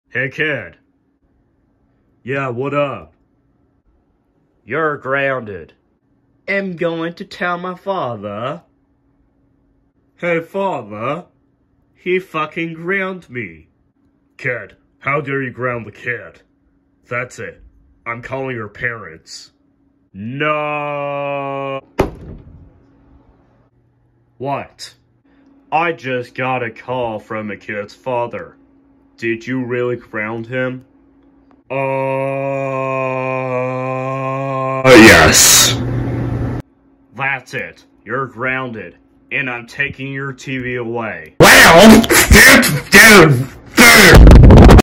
kid grounds kid and gets grounded be like remake
It’s been a year and 2 in a half quarters since i made that thing so I did a remake of it I guess, you guys prefer the original or the remake?